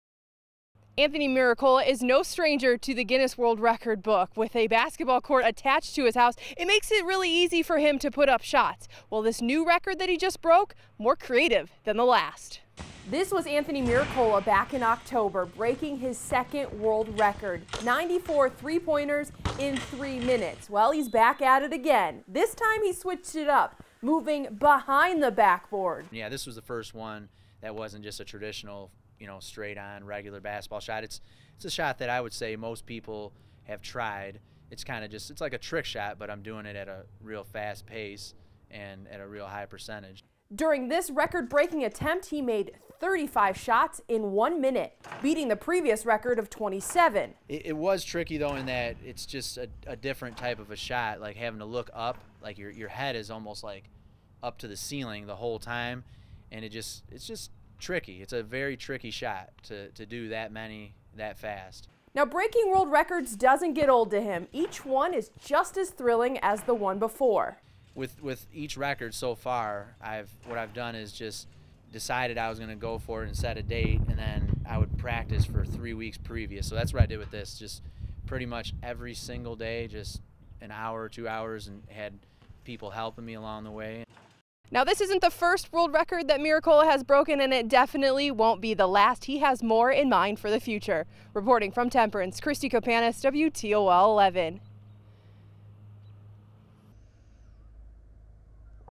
News segment for "Most Behind-The-Backboard Basketball Shots In One Minute."
This is a nice segment pertaining to my third, most recent Guinness World Records title.